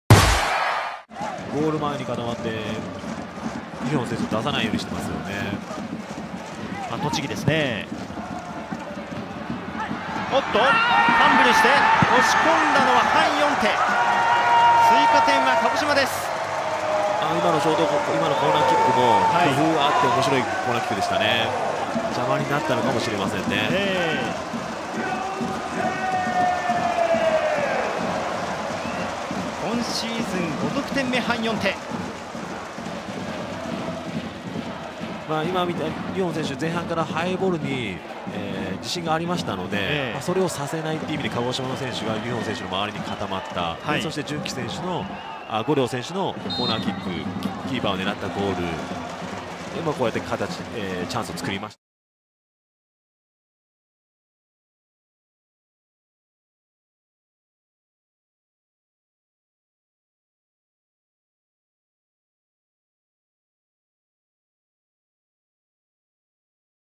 【公式】ゴール動画：韓 勇太（鹿児島）70分 鹿児島ユナイテッドＦＣvs栃木ＳＣ 明治安田生命Ｊ２リーグ 第19節 2019/6/22
CKのチャンスから緩やかなボールが蹴り込まれると、韓 勇太（鹿児島）が相手GKのファンブルを逃さず追加点を奪取！明治安田Ｊ２のゴール動画を公開中！
 2019年6月22日（土）に行われた明治安田生命Ｊ２リーグ 第19節 鹿児島vs栃木のゴール動画です。